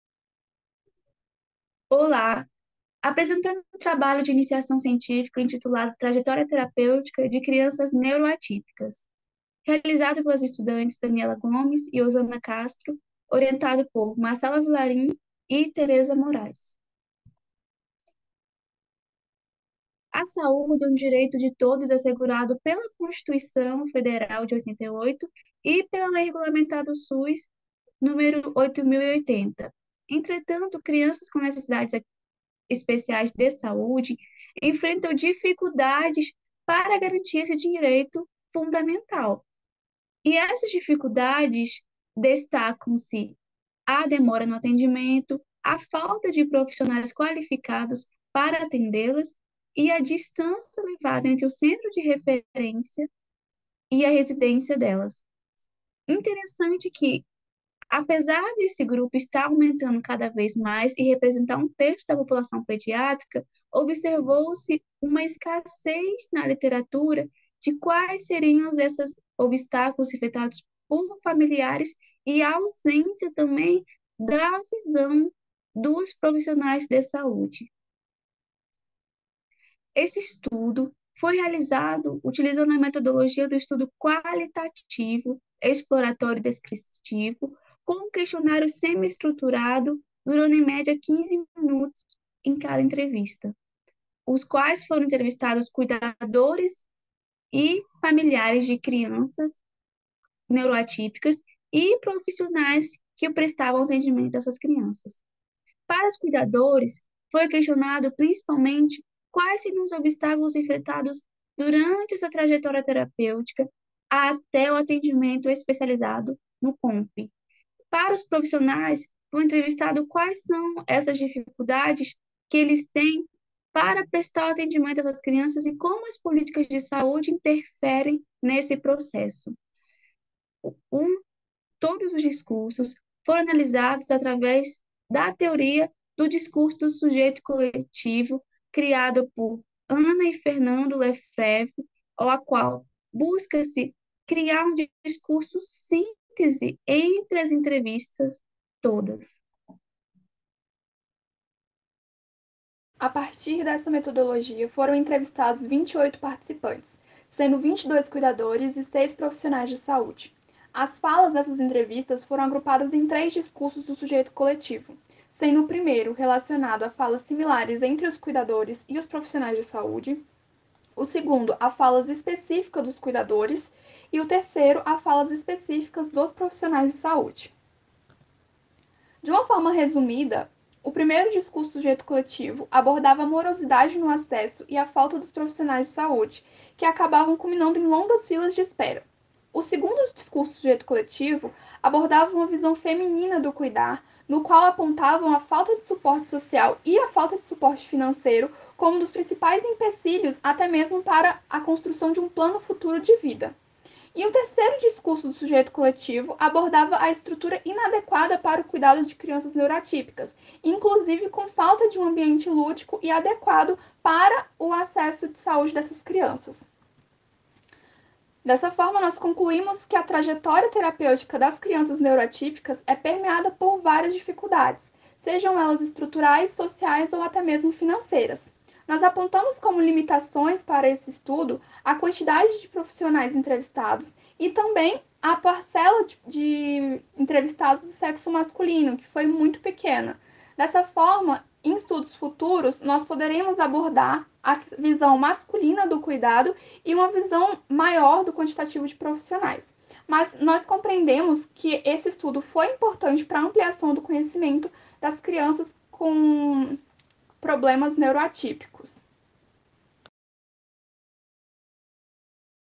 64f4c17e2008f00bcb80766e
Trajetória Terapêutica de Crianças Neuroatípicas: Limites e possibilidades na percepção do
PIBIC - ESCS: Trajetória Terapêutica de Crianças Neuroatípicas: Limites e possibilidades na percepção dos cuidadores e dos profissionais da saúde (id: 64f4c17e2008f00bcb80766e)

Saúde e Vida - Enfermagem
  A saúde é um direito fundamental de todos os brasileiros e dever do Estado, assegurado por meio de políticas sociais e econômicas. Porém, mesmo com as leis que garantem o acesso à assistência, crianças neuroatípicas enfrentam diversos obstáculos em suas trajetórias terapêuticas, derivadas da necessidade de cuidados tecnológicos e medicamentosos, temporários ou permanentes para o seu desenvolvimento. Dessa forma, busca-se, por meio do presente estudo, identificar e analisar os obstáculos que crianças neuroatípicas enfrentam para o acesso e o atendimento nos serviços de saúde do Distrito Federal.

 Estudo qualitativo, exploratório e descritivo realizado com cuidadores e profissionais de saúde. A coleta de dados foi realizada através de um questionário semi-estruturado, tendo por base as seguintes indagações em relação aos cuidadores: quais as dificuldades encontradas para chegar até o atendimento especializado? Como está sendo cuidar de uma criança com necessidades especiais? Quais dificuldades têm enfrentado durante esse processo?
 Os profissionais de saúde foram questionados quanto às dificuldades enfrentadas para prestar assistência à criança neuroatípica e de que maneiras as instituições, os profissionais e as políticas públicas podem auxiliar na melhoria a tais pacientes.
 A análise interpretativa do conteúdo procedeu segundo a teoria do Discurso do Sujeito Coletivo, proposta por Fernando Lefevre e Ana Maria Lefevre, que consiste em tabular os dados em Expressões chaves (ECH), Ideias Centrais (IC) e Ancoragens (AC), através de procedimentos sistemáticos e padronizados.

 A amostra foi composta por 28 participantes, destes, 22 (p= 78,6%) eram cuidadores e 6 (p=21,4%) eram profissionais. Com relação aos cuidadores, a maioria eram mulheres (p=90,9%), desempregadas (63,6%) e que relataram não possuir redes de apoio (p=59,1%). O grupo de profissionais de saúde foi composto por 1 médico, 1 enfermeiro, 1 técnico de enfermagem, 1 psicólogo, 1 fonoaudiólogo e 1 assistente social.
 Nas respostas dos entrevistados, empregou-se as etapas metodológicas do Discurso do Sujeito Coletivo, resultando em 3 discursos: o primeiro discurso é composto por expressões que revelam como a morosidade no acesso é resultado do quantitativo insuficiente de profissionais, atrelada a alta demanda por atendimento especializado. No segundo, referente aos cuidadores, destacou-se a deficiência do suporte social ao enfrentamento de desafios emocionais e financeiros. Por fim, o terceiro discurso engloba falas dos profissionais acerca da estrutura física inadequada para o atendimento.

 Através dos depoimentos dos cuidadores e dos profissionais, nota-se que a trajetória terapêutica de crianças neuroatípicas é permeada por dificuldades no acesso à saúde devido à falta de recursos humanos e estruturais. Além disso, observam-se também dificuldades econômicas relacionadas à falta de mecanismos sociais para a inclusão de cuidadoras no ambiente profissional.
  Compreender tais empecilhos vivenciados por profissionais e cuidadores é primordial para a formulação de políticas públicas efetivas, abrangendo as possíveis vulnerabilidades. Dessa forma, acredita-se que este estudo tenha contribuído para a ampliação da literatura referente a crianças neuroatípicas.
 Entende-se que uma das limitações do estudo foi o número de profissionais entrevistados, porém, encara-se como um estímulo para trabalhos complementares futuros.